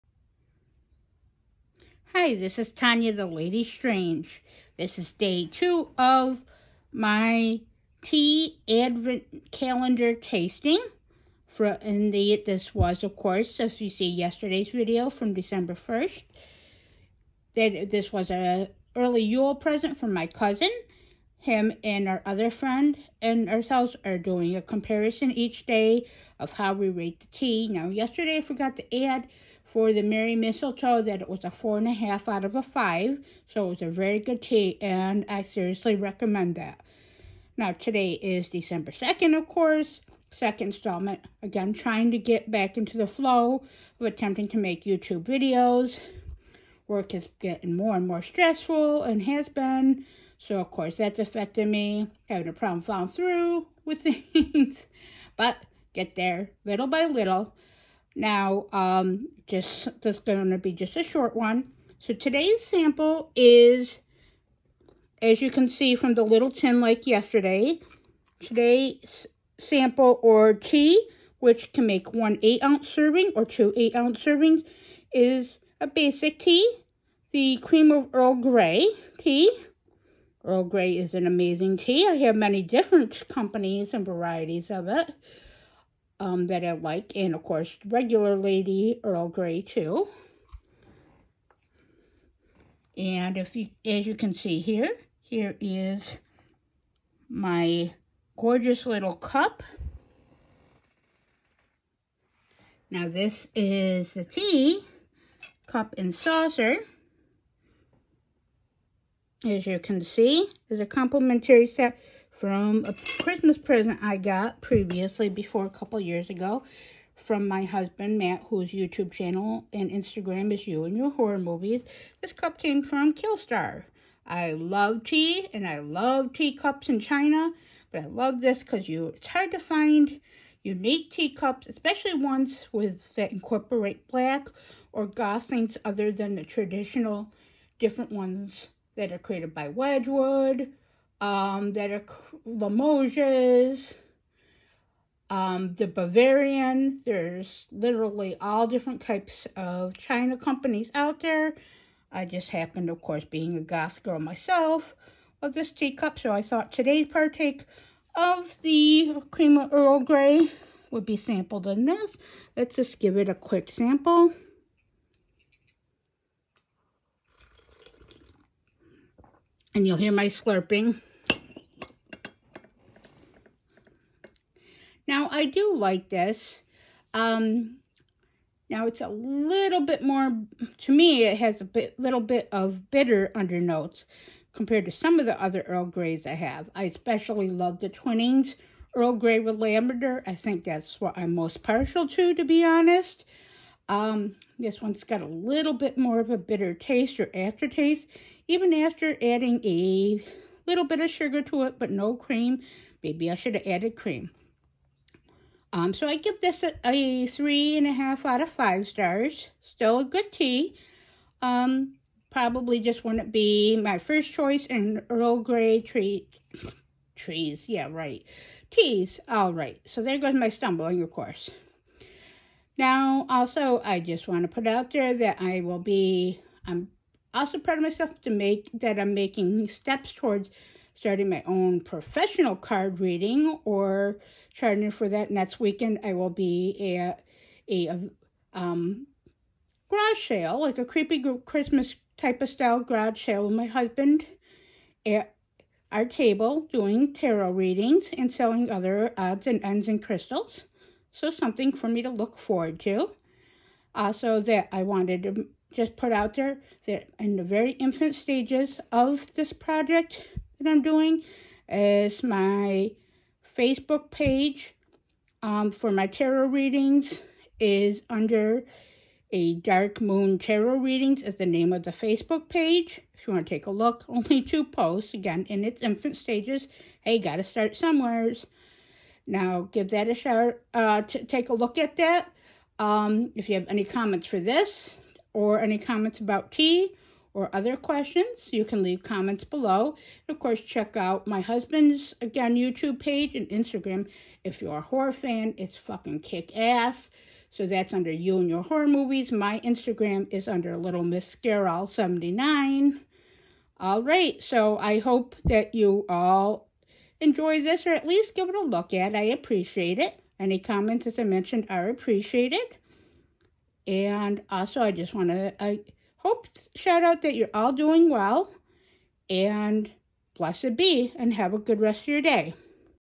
Tea Advent Calendar -Day 2
December 2, 2nd day of tea sampling, Cream of Earl Grey. Cup from killstar!